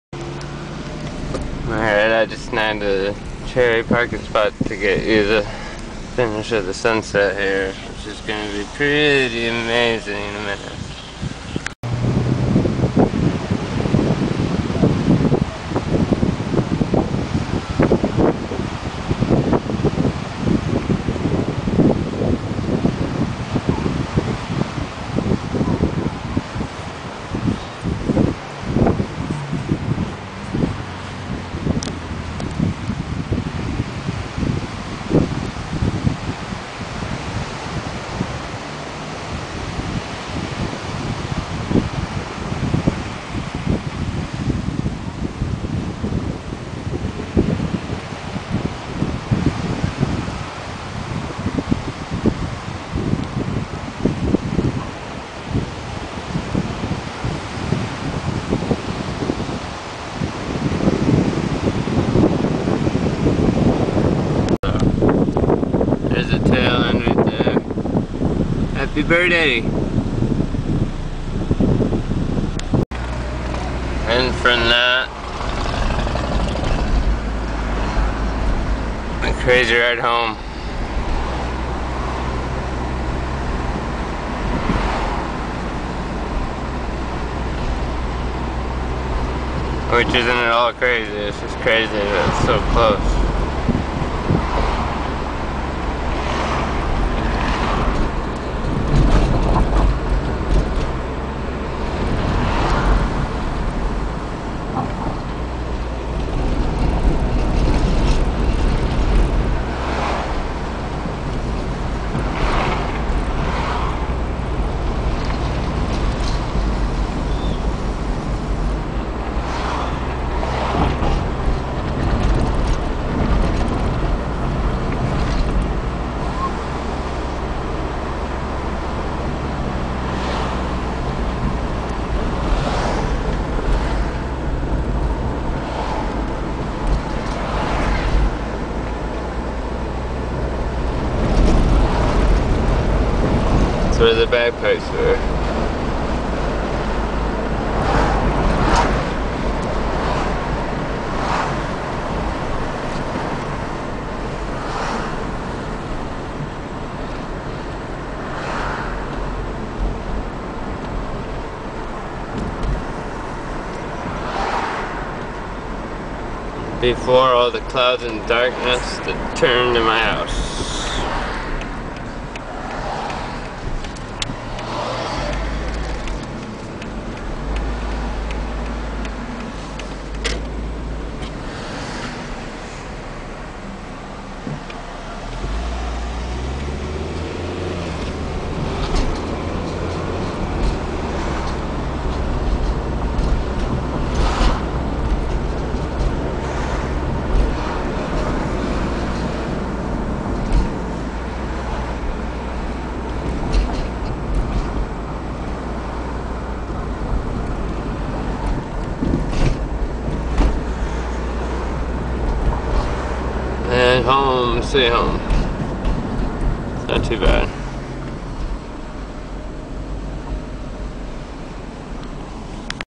DB BDAY 2010-05-20 Part 2
Driving during sunset, Kihei, Maui, HI - May 20, 2010